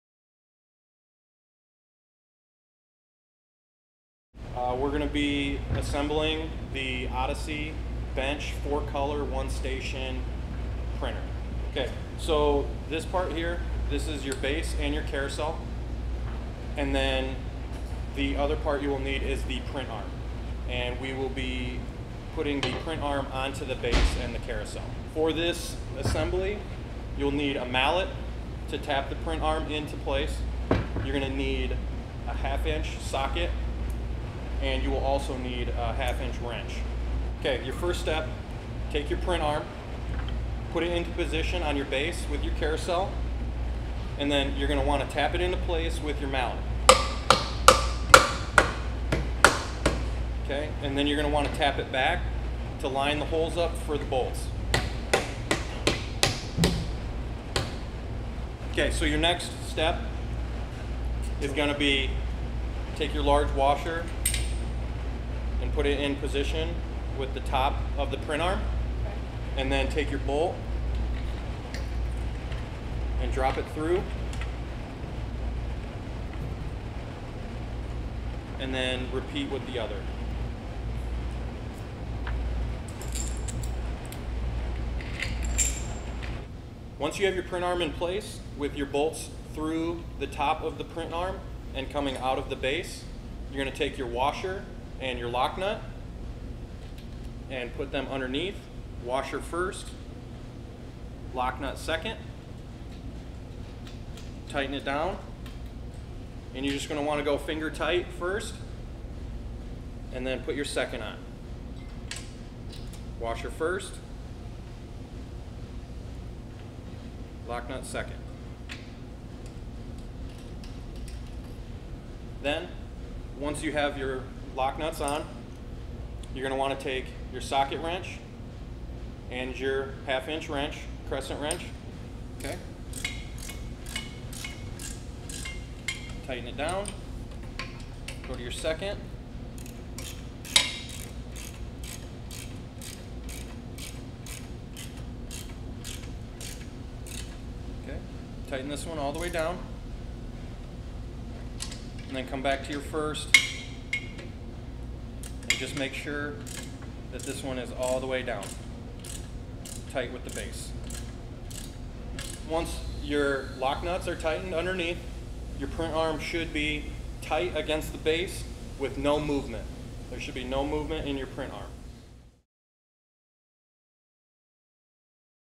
Odyssey Bench Top Model - Installing Print Arm
The step-by-step process of installing the print arm onto an Odyssey Bench Top Model.